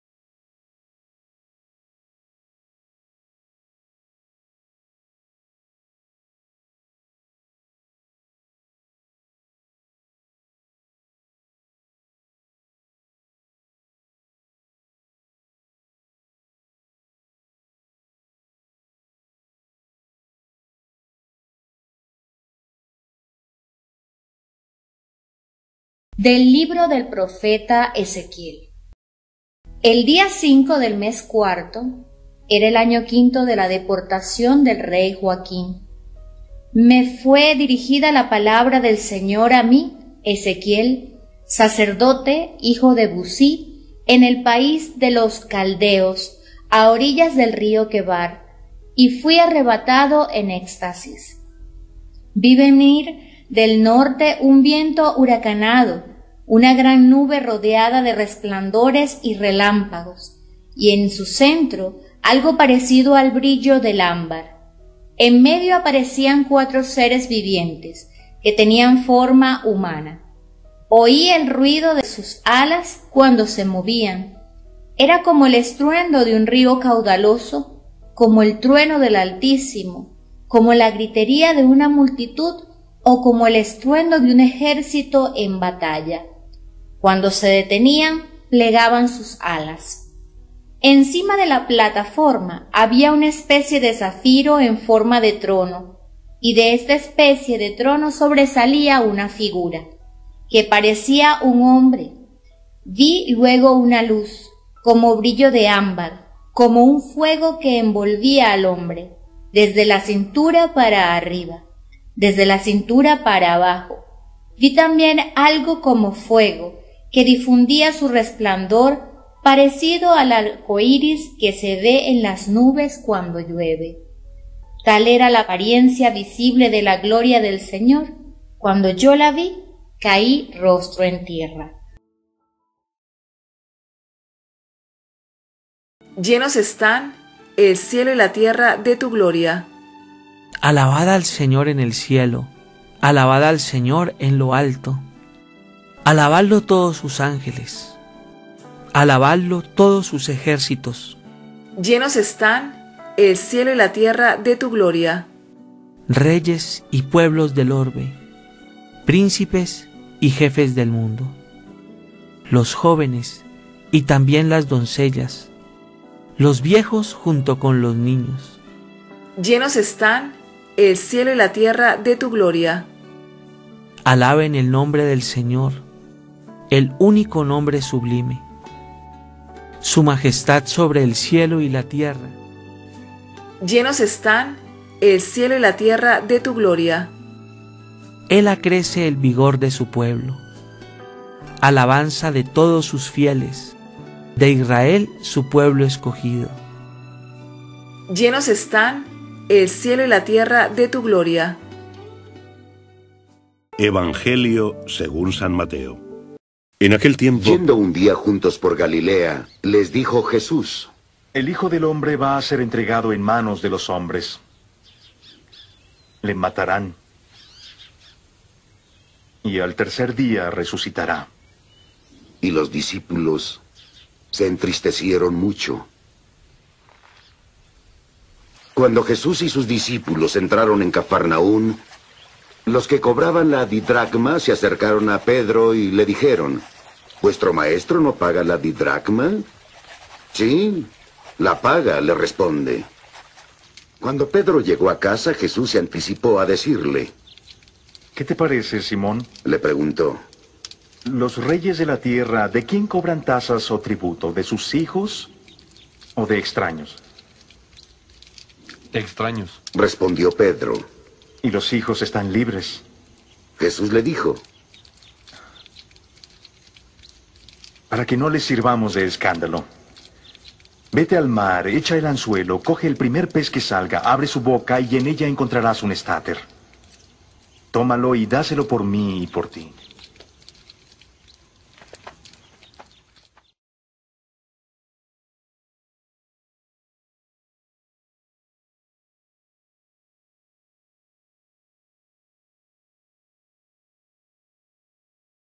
EVANGELIO LUNES 12 DE AGOSTO DE 2024 | MATEO 17,22-27 | LUNES 19°SEMANA TIEMPO ORDINARIO
EVANGELIO Y LECTURAS EUCARISTÍA LUNES 12 DE AGOSTO DE 2024 LUNES DÉCIMO NOVENA SEMANA TIEMPO ORDINARIO CICLO B. Primera Lectura: Del libro del profeta Ezequiel 1, 2-5. 24─2, 1a Salmo 148, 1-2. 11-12ab. 12c-14a. 14bcd R/. Llenos están el cielo y la tierra de tu gloria. Del santo Evangelio según San Mateo 17, 22-27.